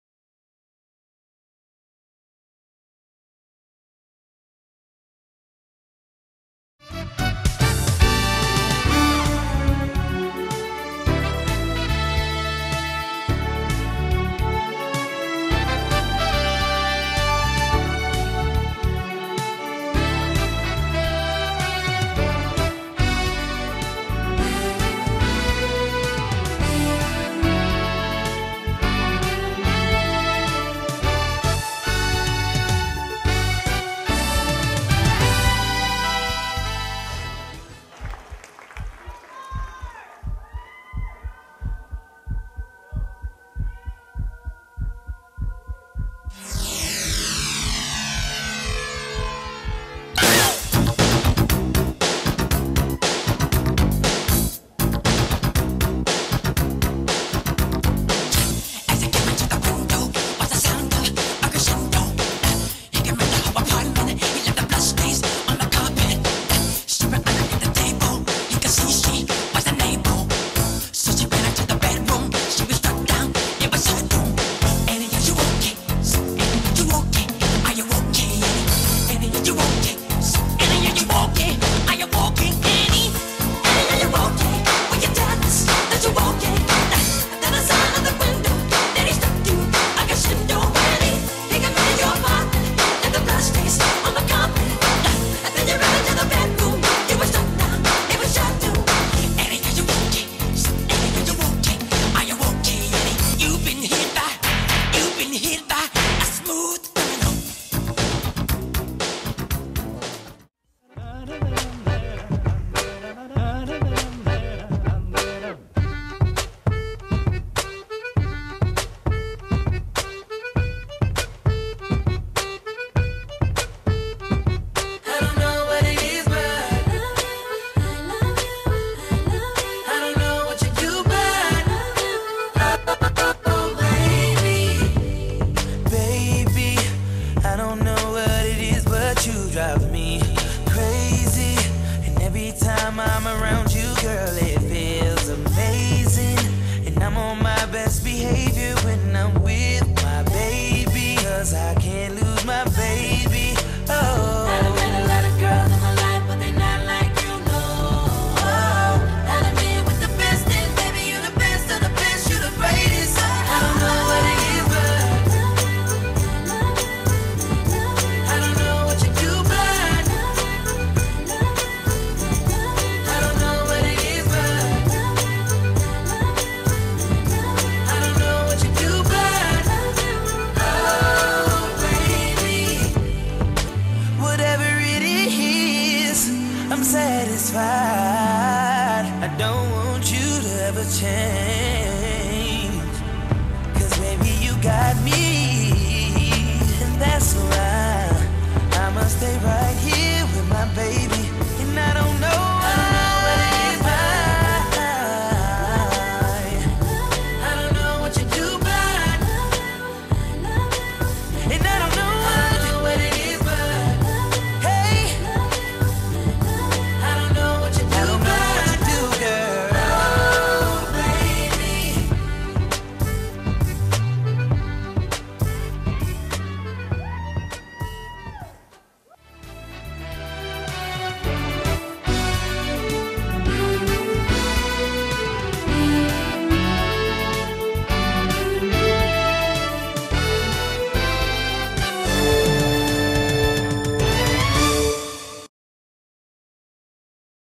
Rainbow Express "Welcome to the Grammy's"
Clip from "Welcome to the Grammy's," the 2010 performance by Rainbow Express Dance Company based in Norco, California.  Rainbow Express teaches jazz, ballet, lyrical, tap and hip-hop to students ages 2 through 18 years of age.